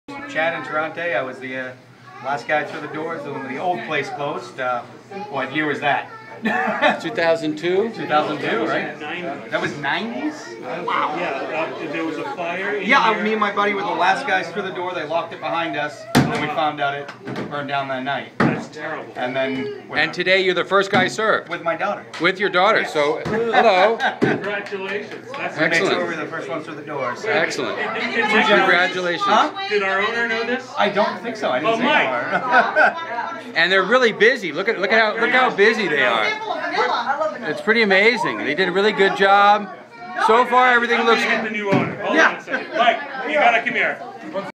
Holy Cow opening - first guy served
This guy was the very last person served at the Holy Cow that burned down in the 90s and the first guy served at the opening.